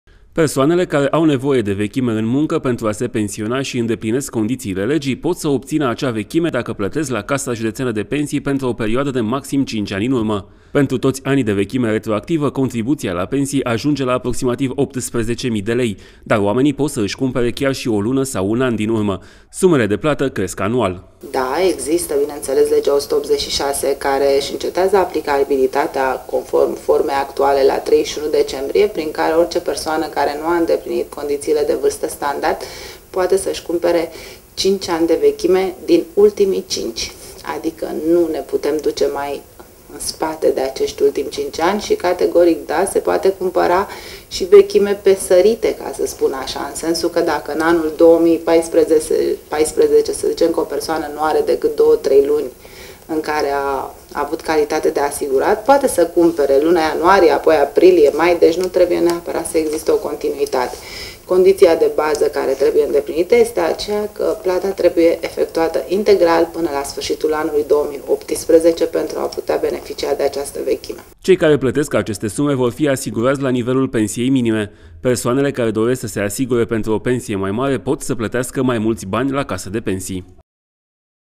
Vechime în muncă, de vânzare
Și anul acesta se poate cumpăra retroactiv vechime în muncă. Persoanele care nu au fost asigurate și nu au ajuns la vârsta standard de pensionare pot cumpăra retroactiv vechime în muncă pentru maximum 5 ani. În județul Dolj, peste 1100 de persoane au cumpărat vechime în muncă în acest an.
  Persoanele care au nevoie de vechime în muncă pentru a se pensiona și îndeplinesc condițiile legii pot să obțină acea vechime dacă plătesc la Casa Județeană de Pensii pentru o perioadă de maximum cinci ani în urmă. Pentru toți anii de vechime retroactivă, contribuția la pensii ajunge la aproximativ 18.000 de lei, dar oamenii pot să își cumpere chiar și o lună sau un an, din urmă. Sumele de plată cresc anual.
  Cei care plătesc aceste sume vor fi asigurați la nivelul pensiei minime. Persoanele care doresc să se asigure pentru o pensie mai mare pot să plătească mai mulți bani la Casa de Pensii.